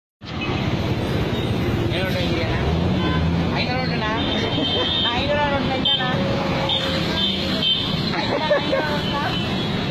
NOT ACCEPTED BY A BEGGER Rs500 note
500 ரூபாய்க்கு பிச்சைக்காரனிடம் கூட மதிப்பு இல்லை ... நாட்டில் 100 ரூபாய்க்கு பஞ்சம் ஒரு ATM இல் கூட 100 ரூபாய் 50 ரூபாய் இல்லை ...அப்புறம் எப்படி ATM  நாங்க பணம் எடுக்குறது ...